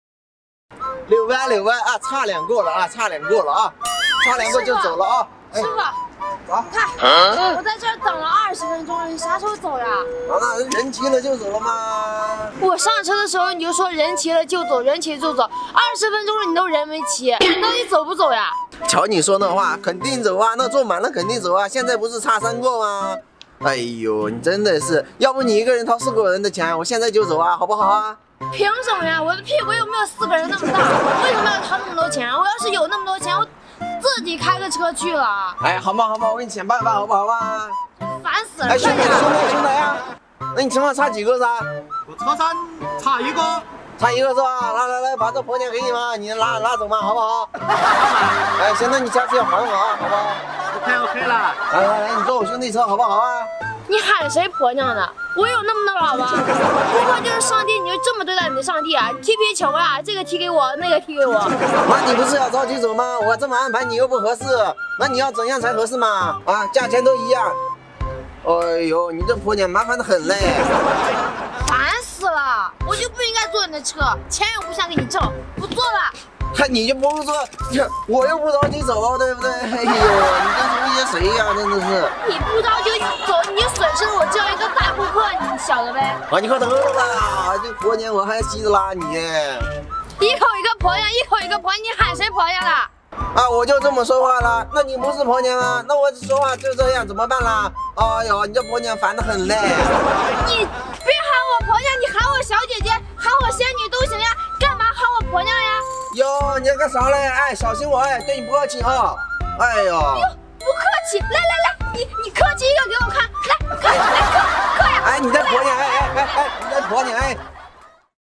美女打车等太长时间，和司机俩人一人一句互怼，过程太搞笑了【二平說】
原創搞笑視頻，盡在二平說